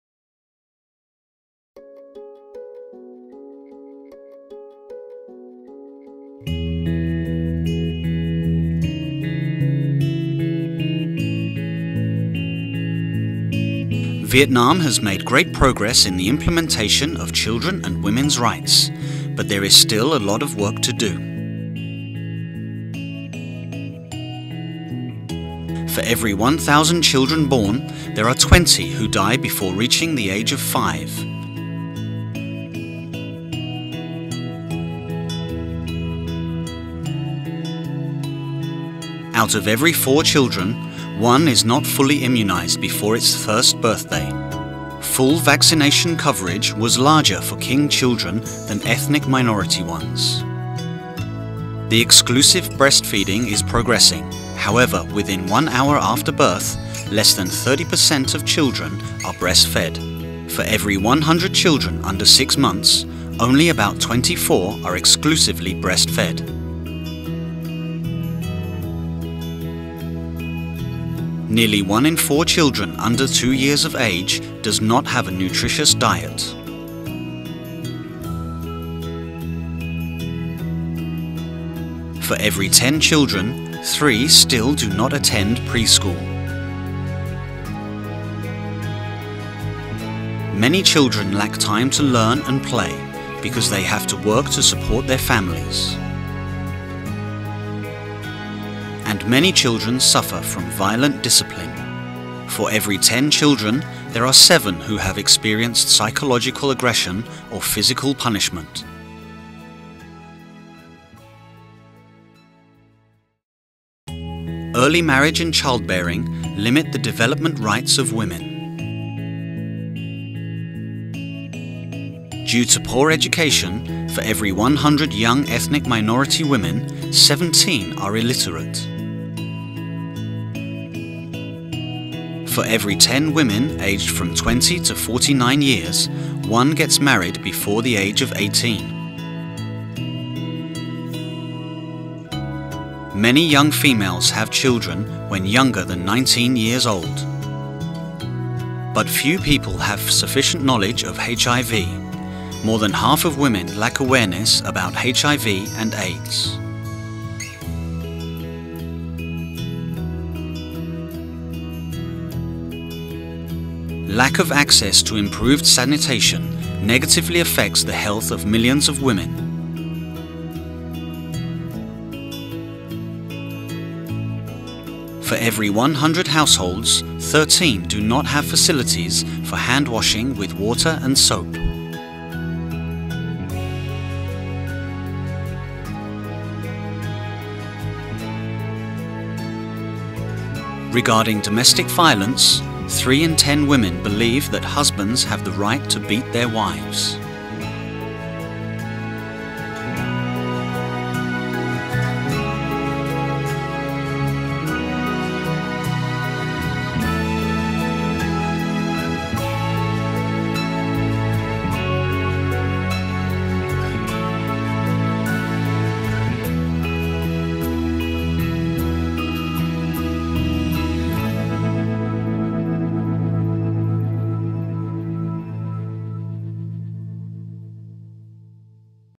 Viet Nam's Multiple Indicator Cluster Survey 2014
Viet Nam's Multiple Indicator Cluster Survey (‪#‎MICS2014‬) launched today in Ha Noi, highlights an unfinished agenda in the lives and well-being of vulnerable children and women while capturing some of the country's progress toward achievement of Millennium Development Goals (MDGs). The MICS survey was carried out by the General Statistics Office (GSO) in close collaboration with various line ministries and with technical and financial support from UNICEF.